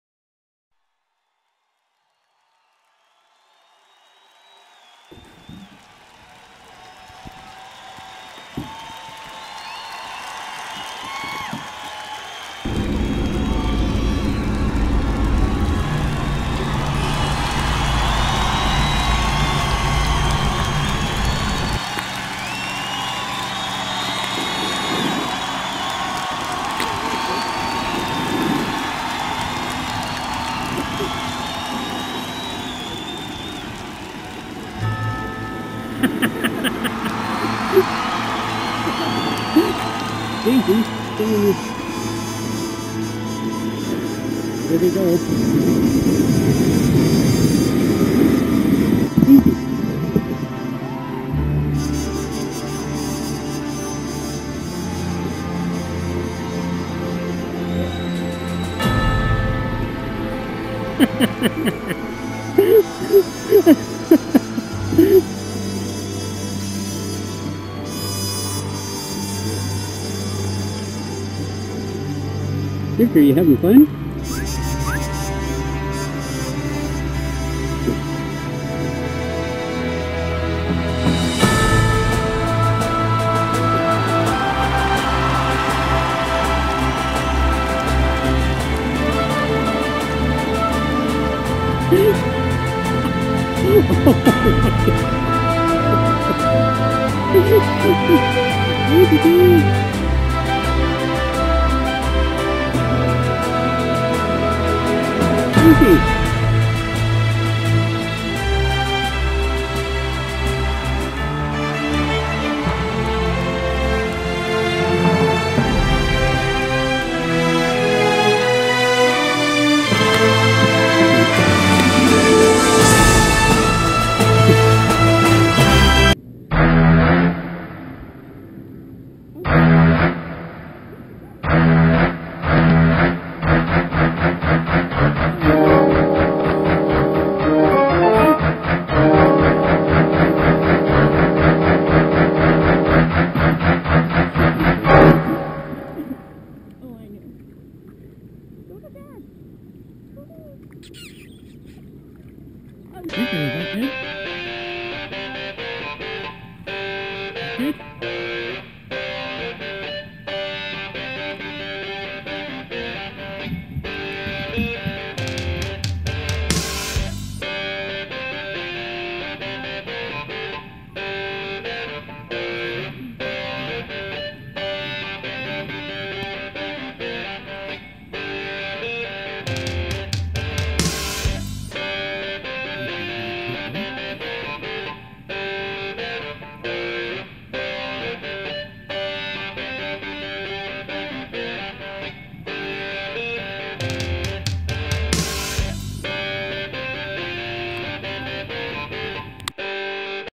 Tucker's First Kayak
Took the dogs out, Tucker's first time.
Sarnia Canatara Beach